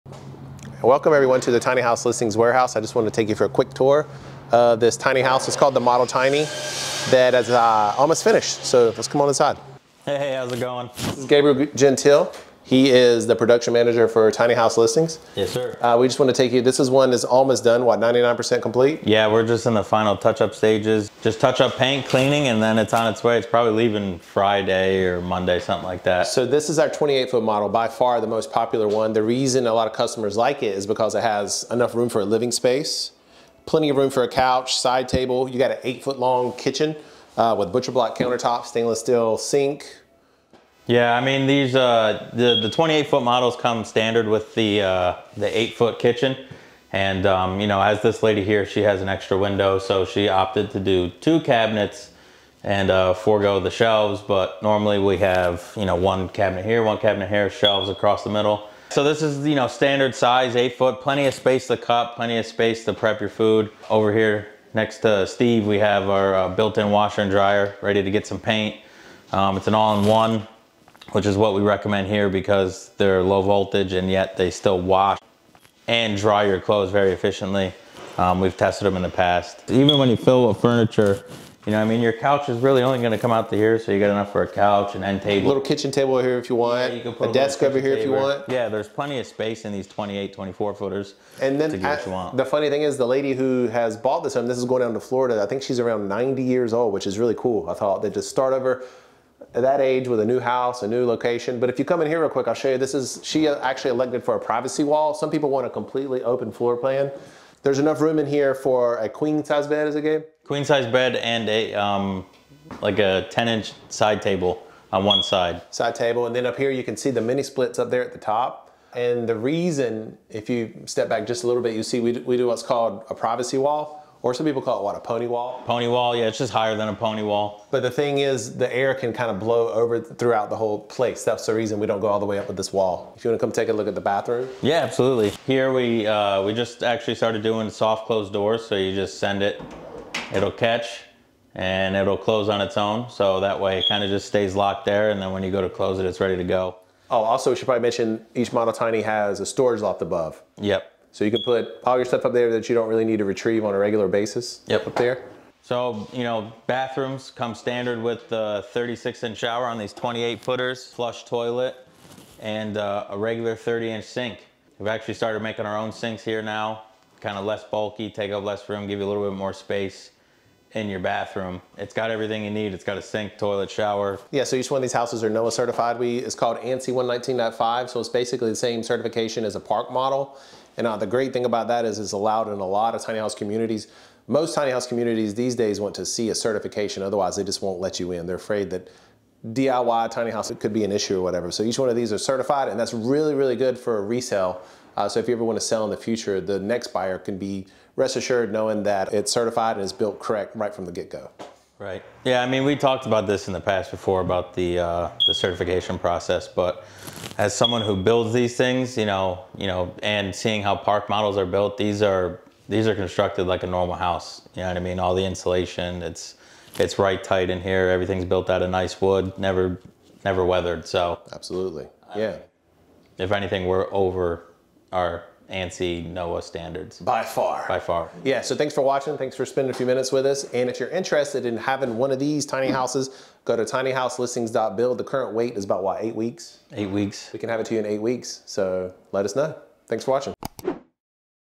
Discover The Model Tiny! 🏡✨
Join us for a grand tour of our 28-foot masterpiece and explore the endless possibilities for customization. Let us build your dream home! 🛋️🏠

Go to Tinyhouselinstings.build to customize your Dream Home✨